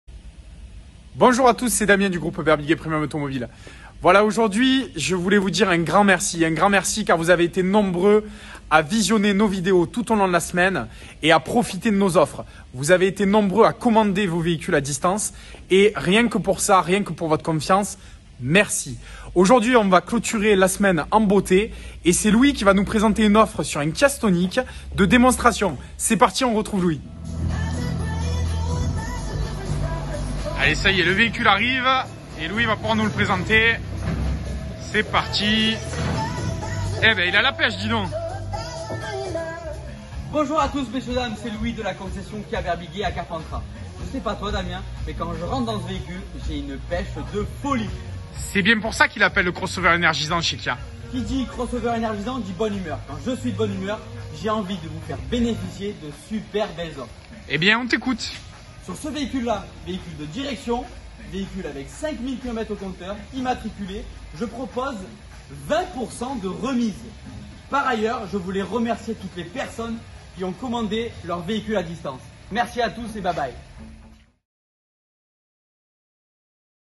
🔥OFFRE SPECIALE CONFINEMENT🔥 20% de remise sur le nouveau Kia STONIC HYBRIDE
💪 Damien et Louis vous invite à commander à distance sur un Kia STONIC HYBRIDE avec 20% de remise 💪
Carte grise gratuite
éligible au bonus écologique
pas de TVS